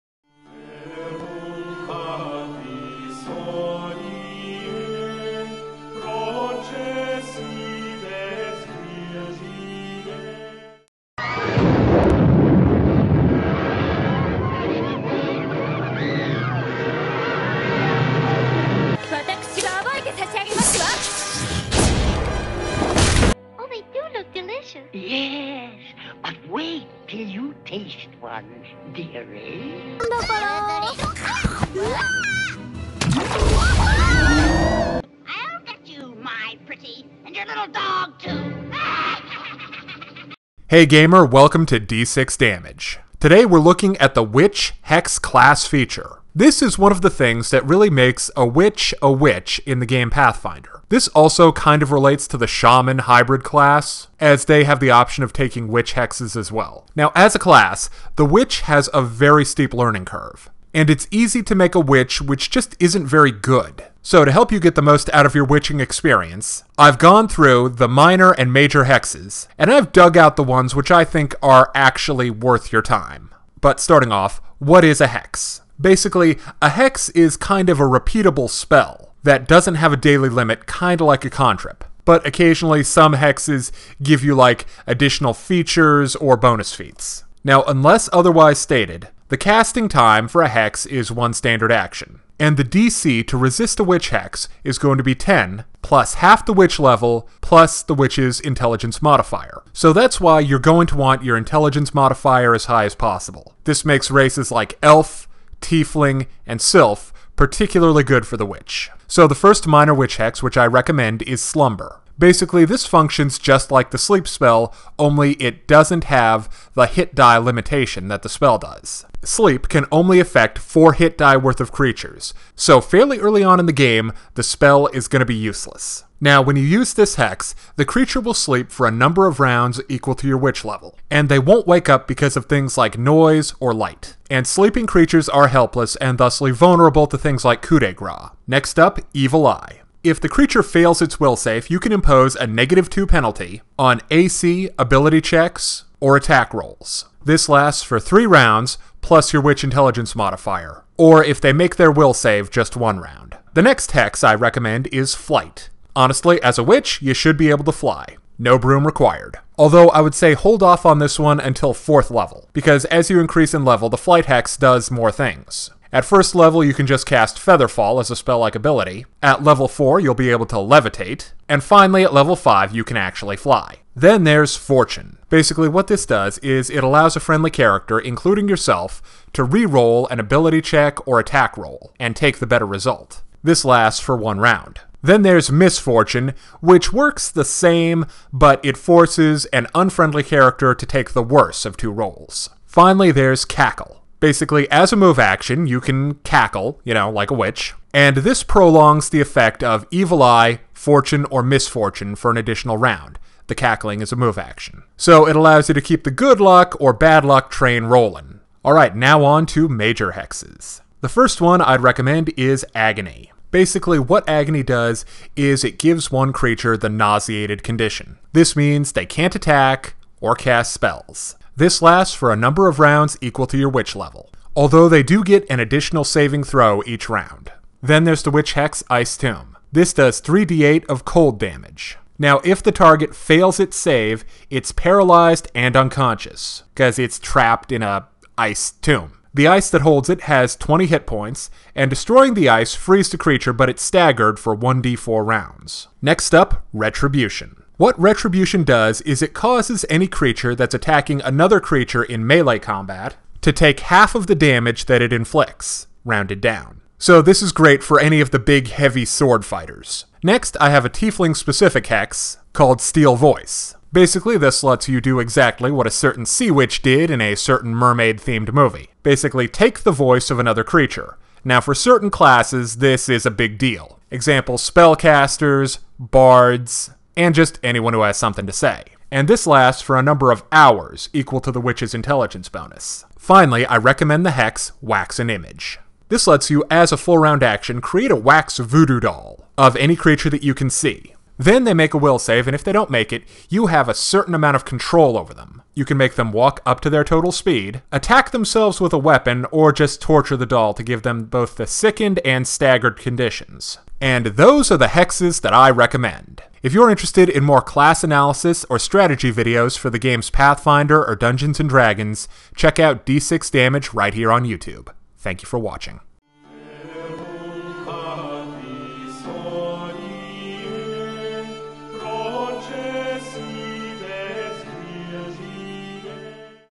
Pathfinder Witch Hex Guide
Welcome to the Pathfinder witch hexes guide. I will explain the witch hex class feature and list the best of the common hexes and major hexes you have at your disposal.